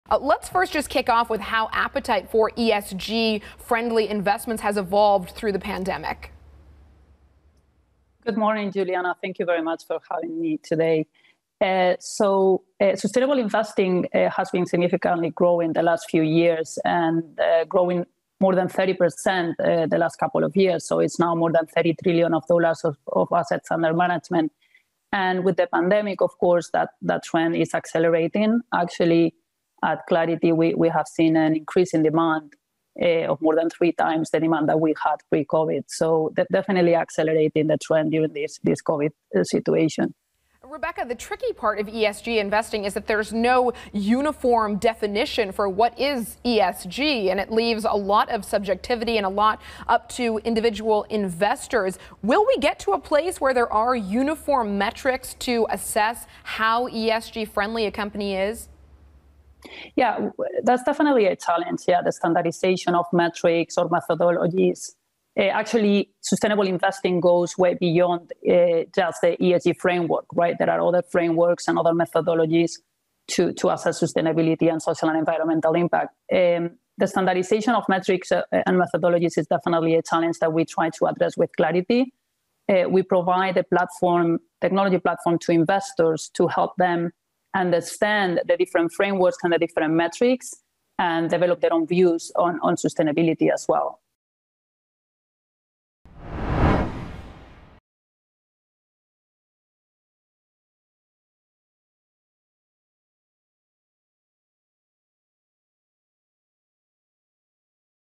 Covid-19 pandemic has accelerated ESG investing trend: CEO
Rebeca Minguela, founder and CEO of Clarity AI, discusses the rising demand for ESG tracking and monitoring and the impact of the Covid-19 pandemic on that trend.